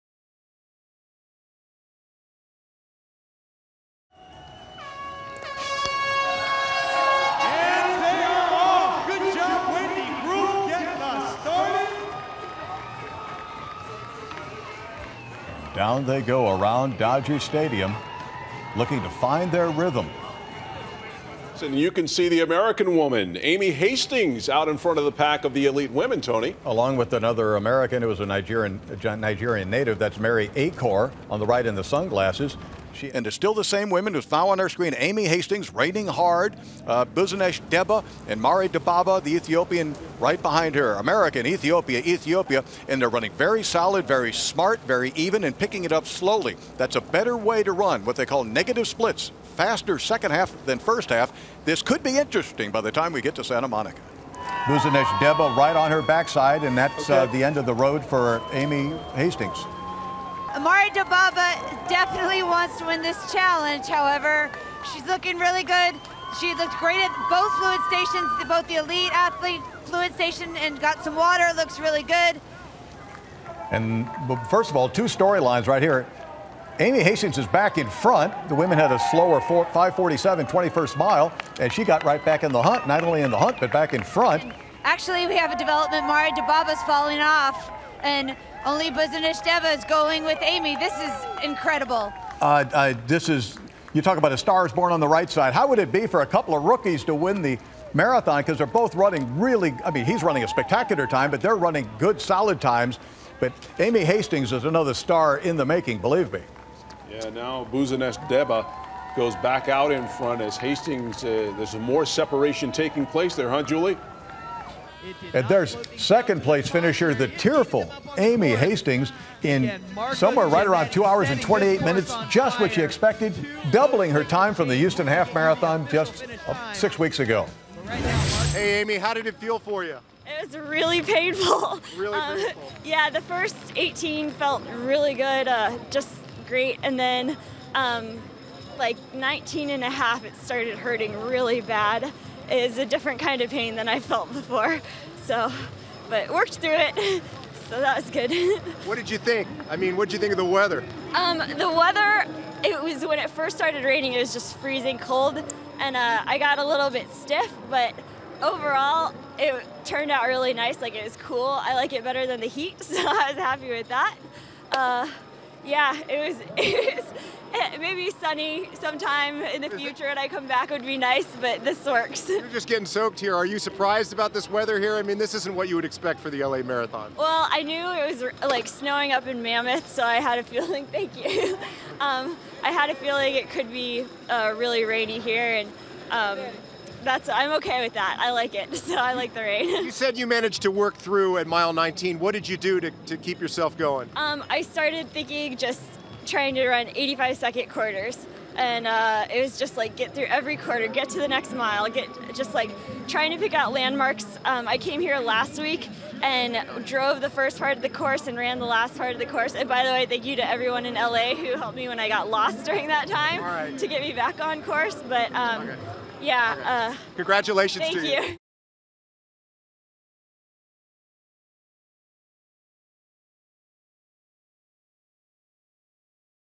Amy Hastings 2nd in LA Marathon - from Universal Sports
2011, Los Angeles Marathon, American Hastings battled back a forth to hold the lead as long as she could, but still managed to cross in second place on the rainy day. (Watch the full event at UniversalSports.com)